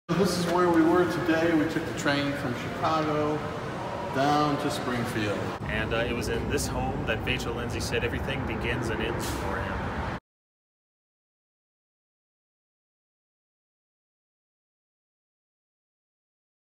The Murals at the Vachel Lindsay house in Springfield IL.
A short clip of one of the caretakers of the Vachel Lindsay house!